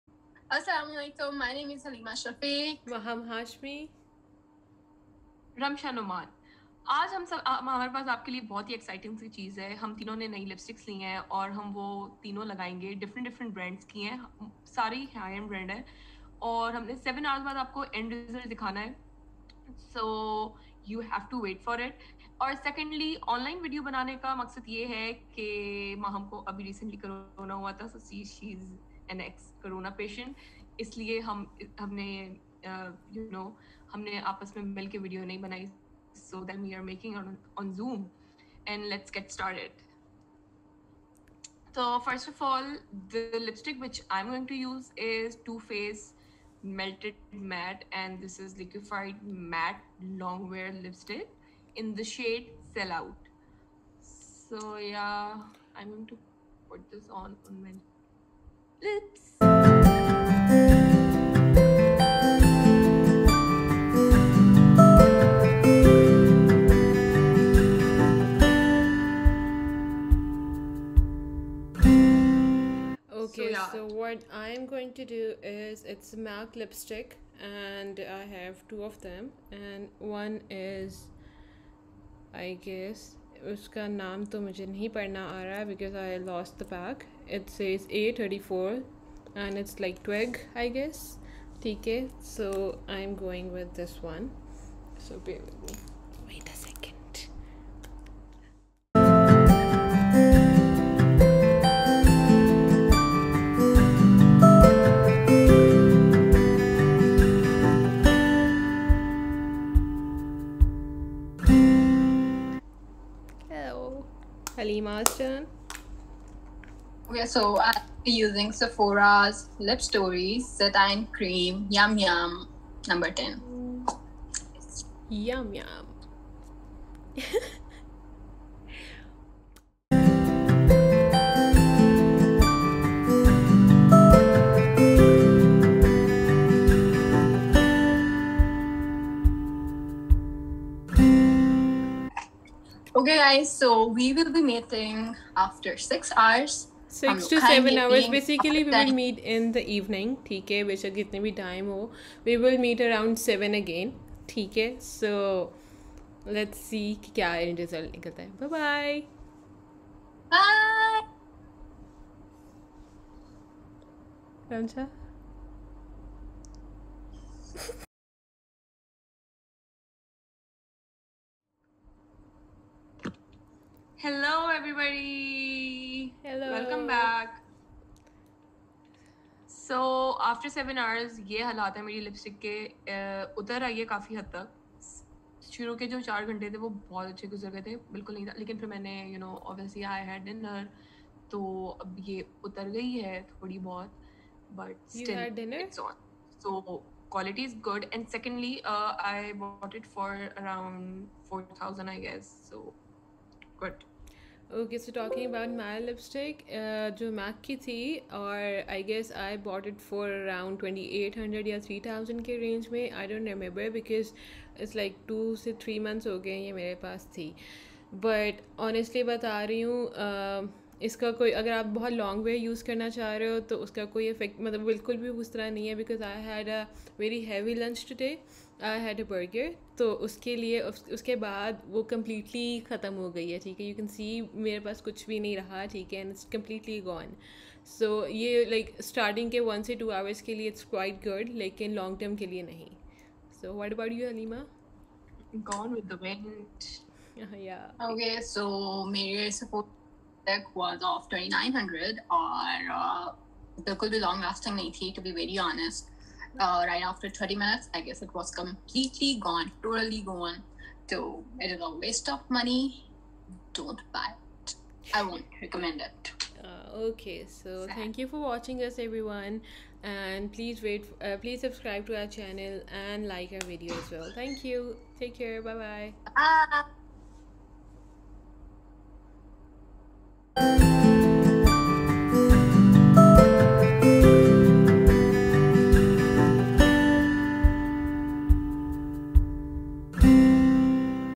Lipstick Review
This week’s video was made by us on zoom meeting since one of our group member recovered from corona recently so we were avoiding to meet. We decided to do a product review, so basically what we did was that three of us chose three different lipsticks of three different high end brands and we applied it on our lips and then we waited for 7 hours and then showed the finals results. We found out that mostly the lipstick was gone because we ate the lipstick lol. Although we applied the high end matte finish lipsticks but still it did not stay on for a very long time. However it was fun filming this video for you all.

Group 15

Ramsha Noman

Haleema Shafique

Maham Shahid Hashmi